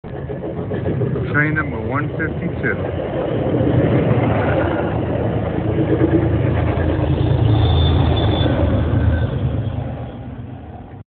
01/18/24 MBTA passenger train no.152 heads to Boston MA with a safety first car on it's train.
MBTA passenger train no. 152 heads to Boston MA with a safety first car on it's train. 01/18/24.